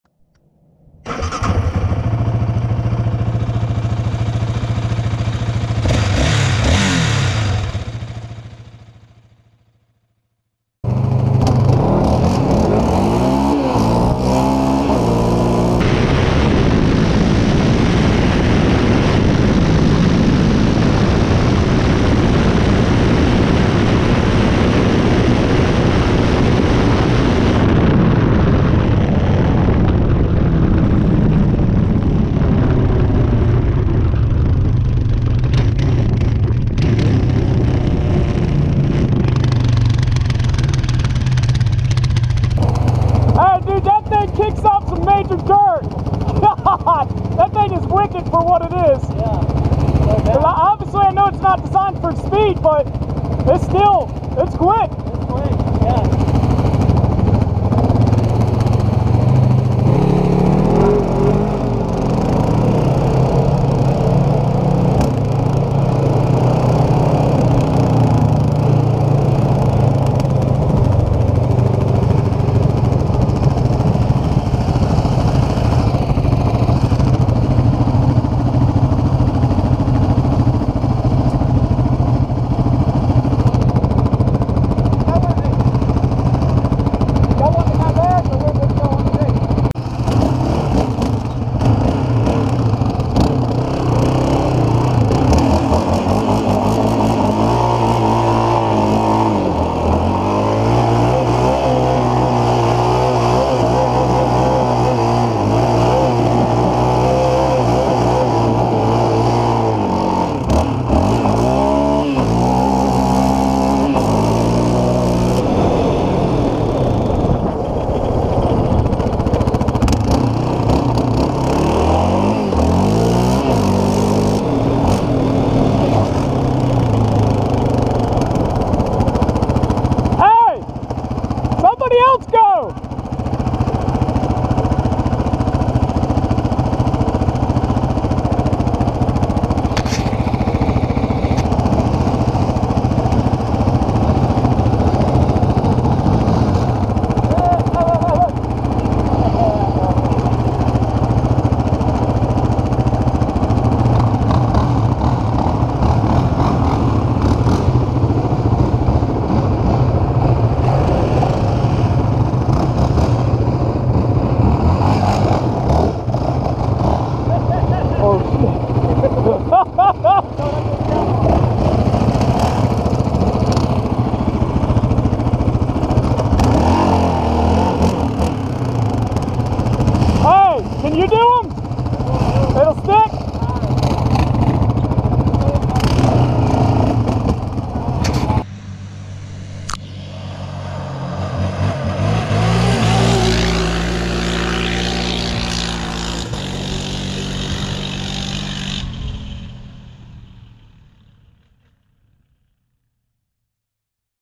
Yamaha Raptor 700R vs Polaris RZR 1000! Plus, buddy almost flips his Raptor!
2018 Yamaha Raptor 700R SE vs 2019 Polaris RZR 1000! The RZR was in 2WD mode with one passenger. Plus, buddy on 2018 Yamaha Raptor 700 almost flips while doing a donut!